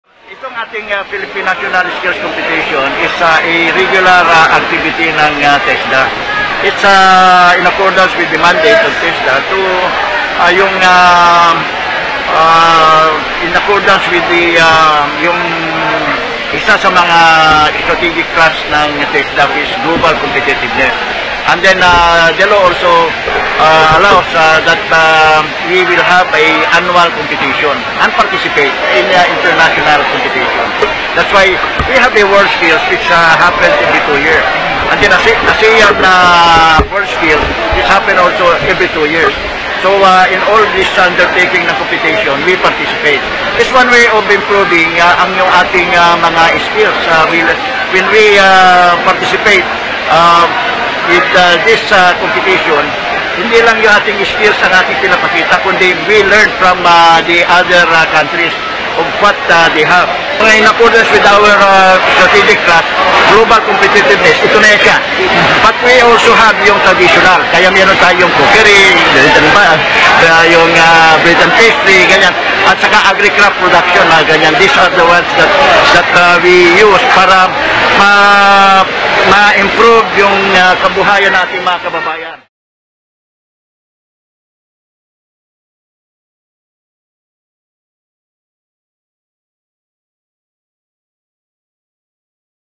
TESDA holds Philippine National Skills Competition 2021
Video by RADYO INQUIRER ON-LINE